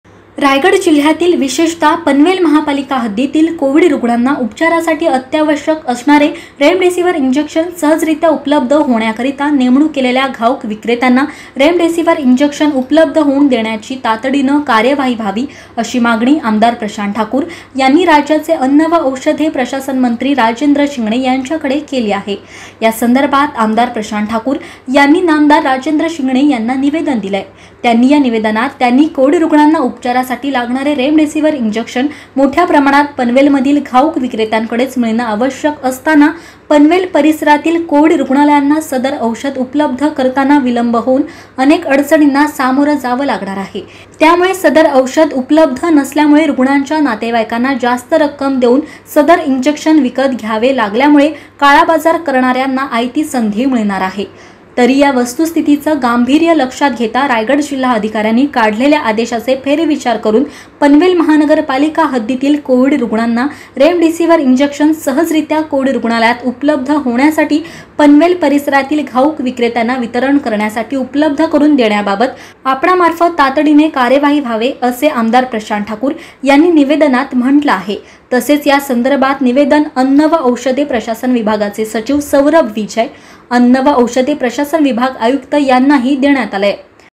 Malhar Tv News रेमडेसिव्हीर इंजेक्शन उपलब्ध करून देण्याची तातडीने कार्यवाही व्हावी !
malhartvnews at panvel | रेमडेसिव्हीर इंजेक्शन उपलब्ध करून देण्याची तातडीने कार्यवाही व्हावी; आमदार प्रशांत ठाकूर यांची अन्न व औषधे प्रशासन मंत्री राजेंद्र शिंगणे यांच्याकडे मागणी